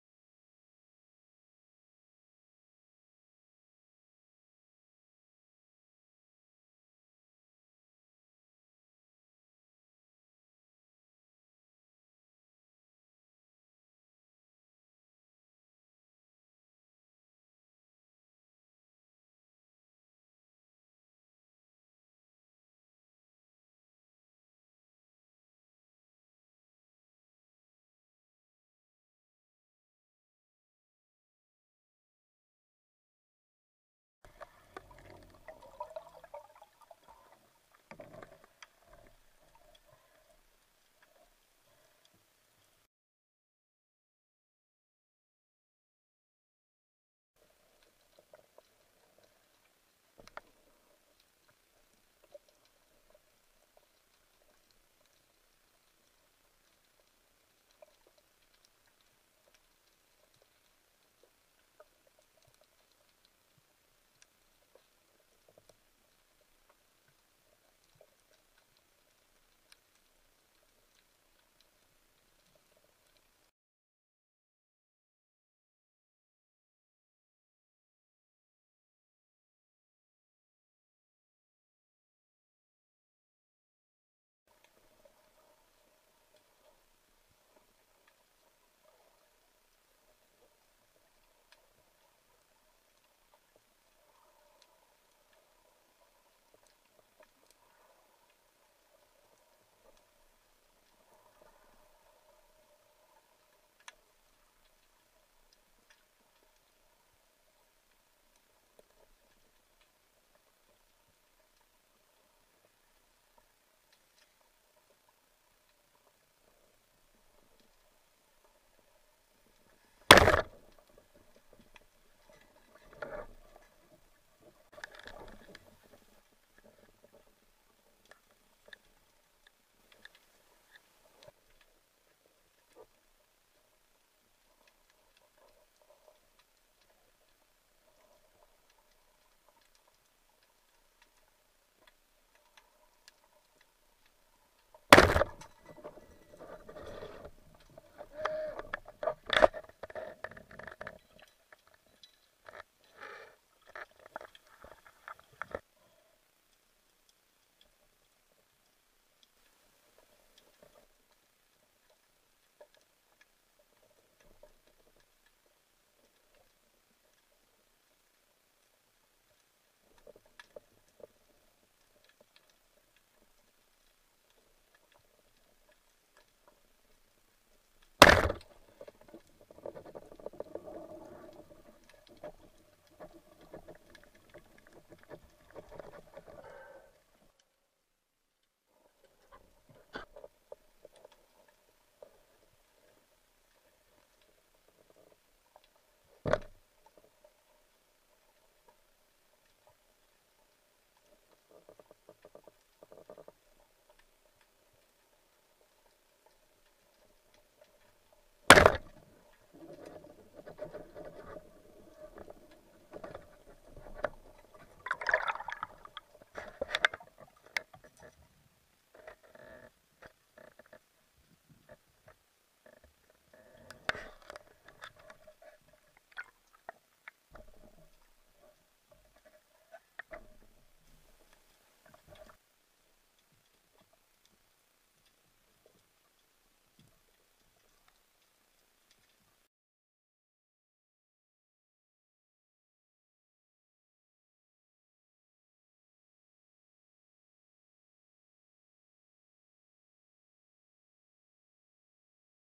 Pescasub: L'erba del vicino...
Quando si va a pesca in un posto nuovo di cui ti hanno detto meraviglie finisce sempre così...